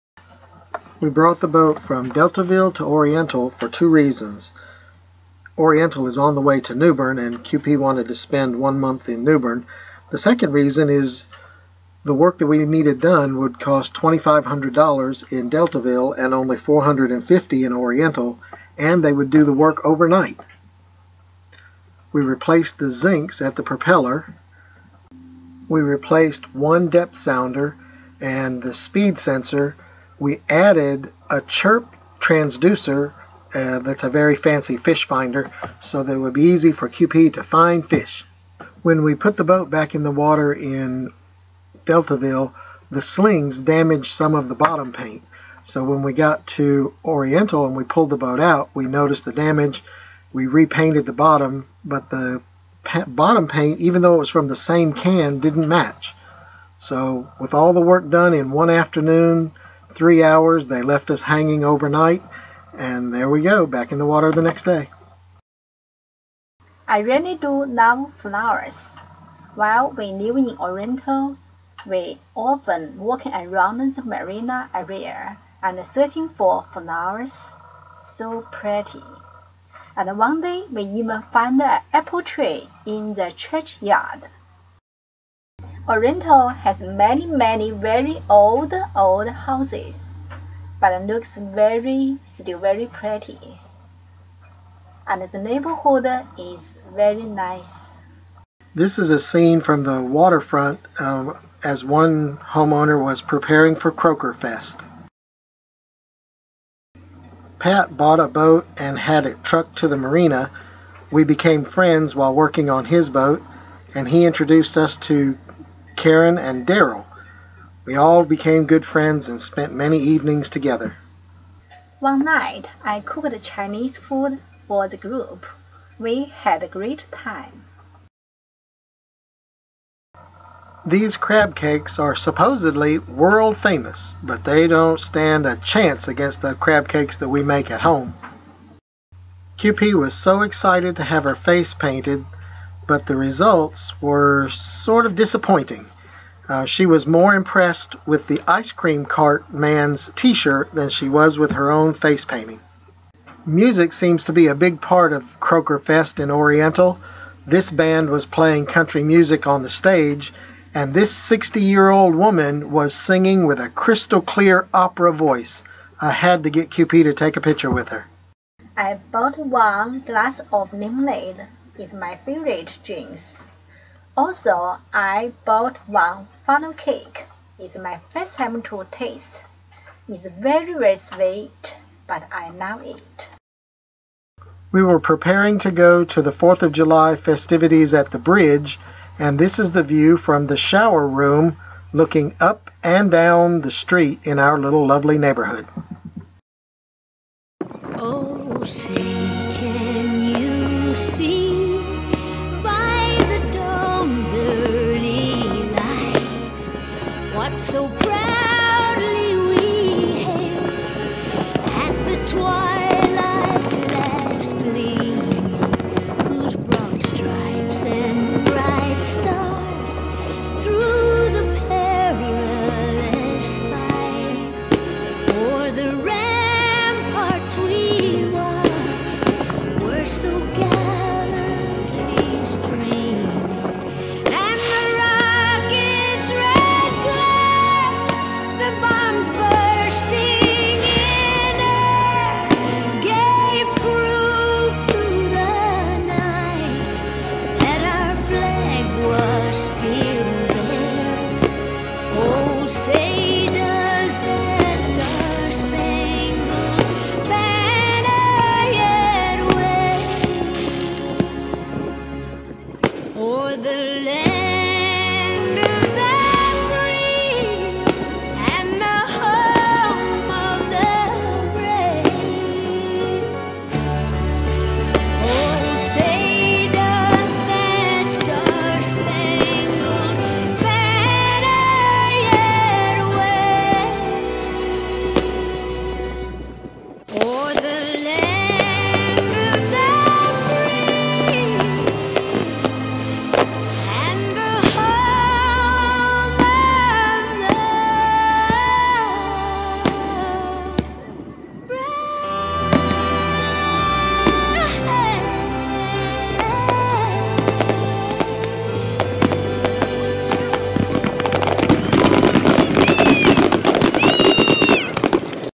Living Aboard Freya 2015 07 02 to 20 While in Oriental, NC
We stayed in Oriental, NC for a month to work on Freya and also to give her a safe place to hang out while QP and I went to China.